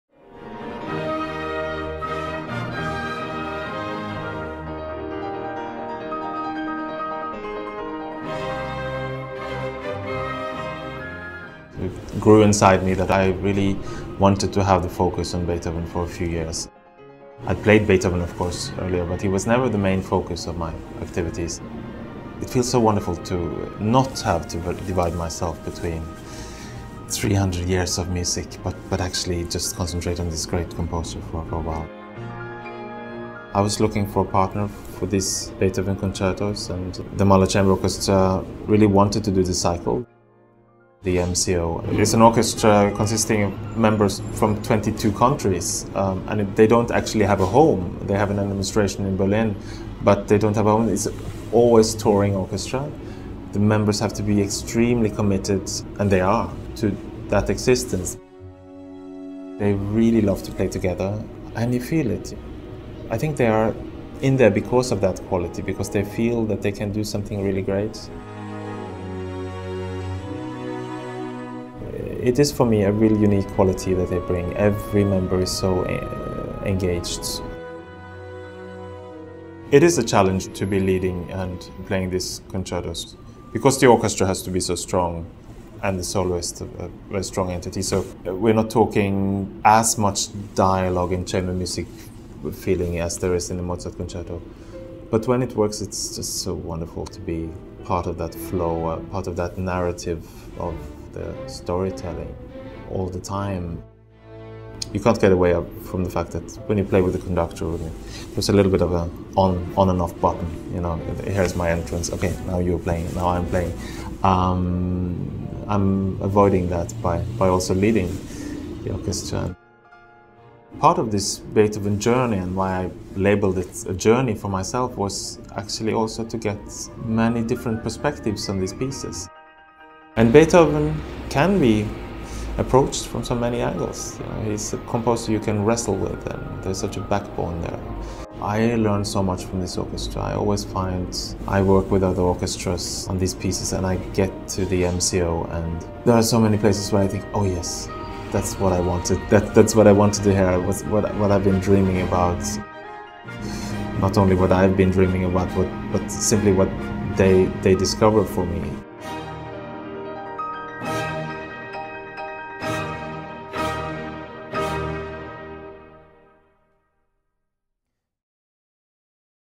Leif Ove Andsnes and The Beethoven Journey
Pianist Leif Ove Andsnes discusses The Beethoven Journey and his partnership with the Mahler Chamber Orchestra.